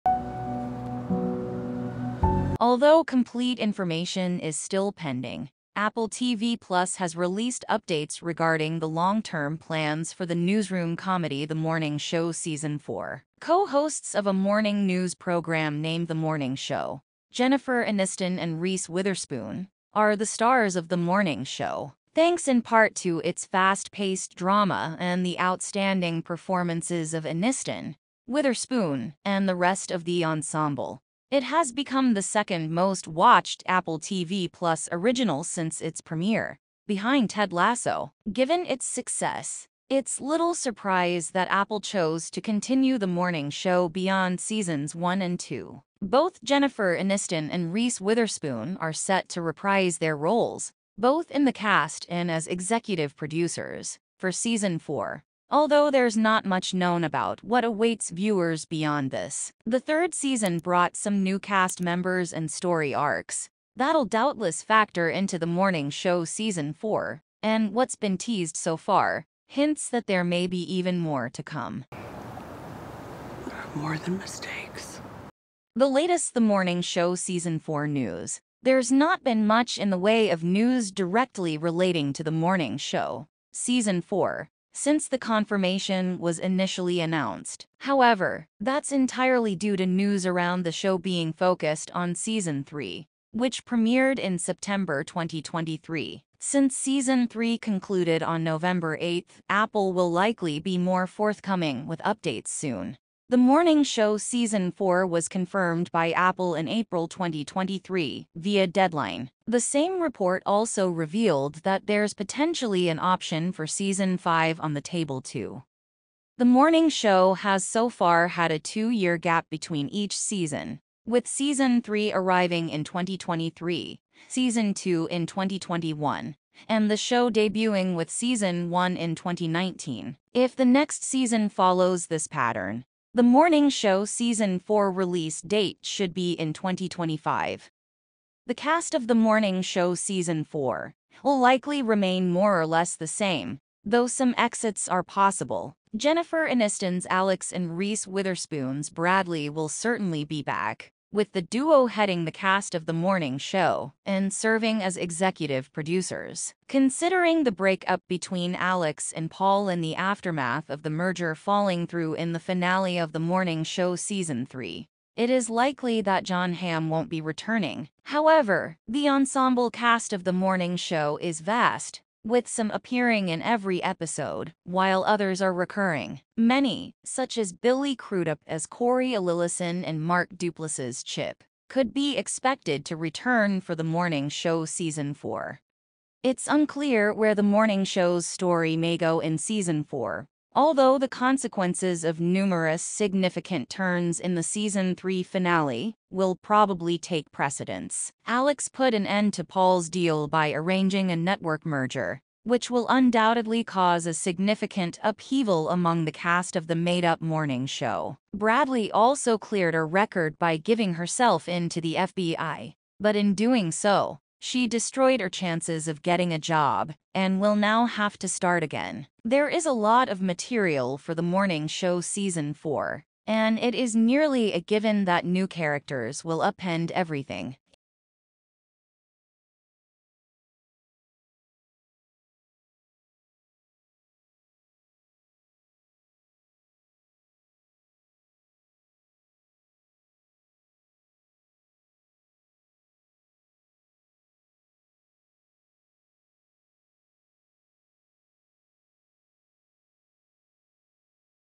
Everything We Know About The Morning Show Season 4
Everything We Know About The Morning Show Season 4. When will The Morning Show season 4 air? A premiere date has not been announced, but if the current pattern of releasing a season every other year continues, we're looking at a 2025 release for season 4.